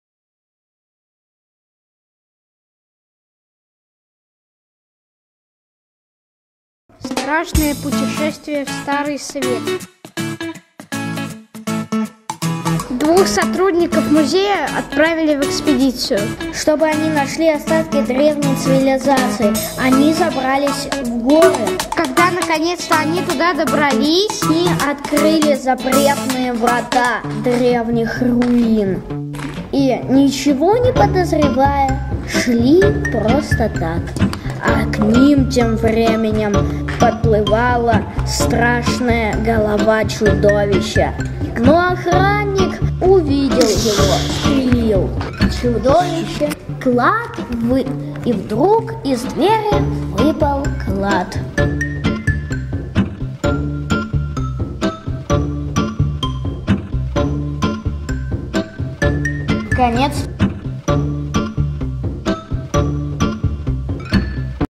Страшное путешествие в Старый свет
Мультфильм создан на занятиях по lego-анимации в Детском центре при Еврейском музее.
Авторы Зайцев Артемий, Оверченко Кирилл
Мастер Наталья Ивлева

февраль 2015 год